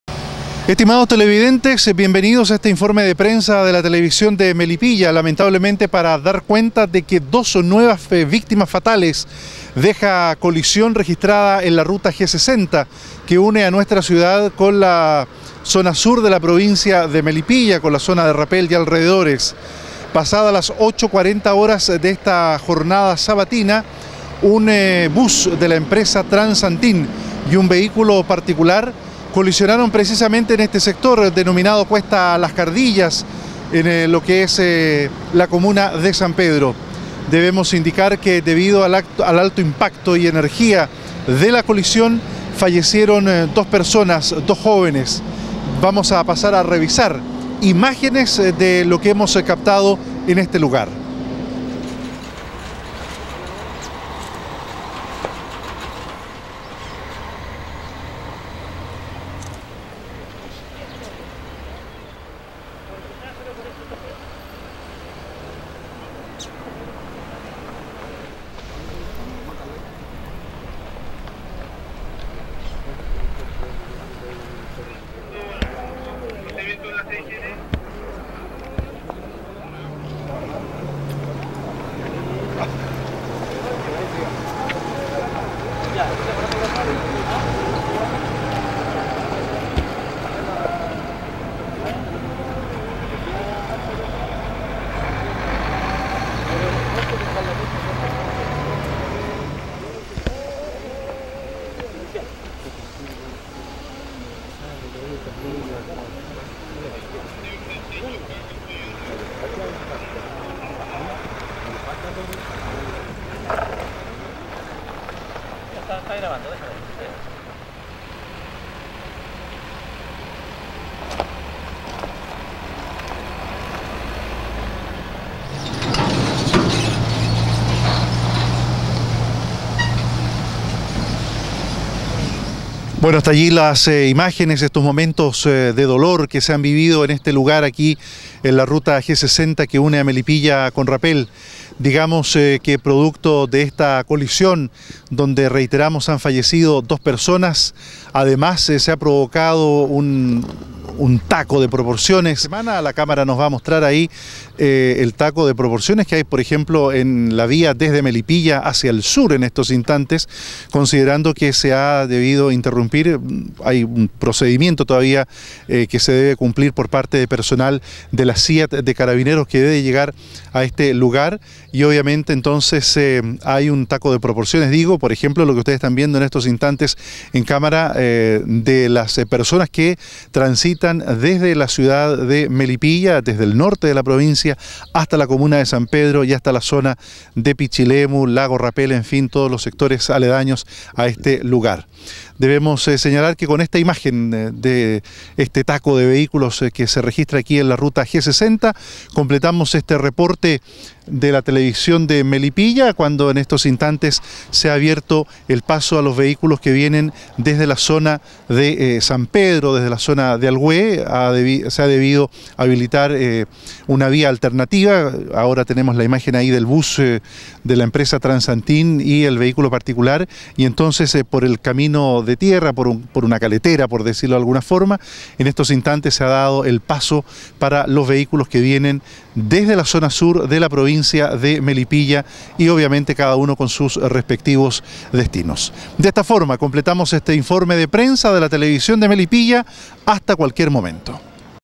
17 ENERO 2015 DOS JOVENES FALLECIDOS EN CHOQUE CON BUS EN RUTA MELIPILLA RAPEL
DOS JOVENES FALLECIDOS EN CHOQUE CON BUS EN RUTA MELIPILLA RAPEL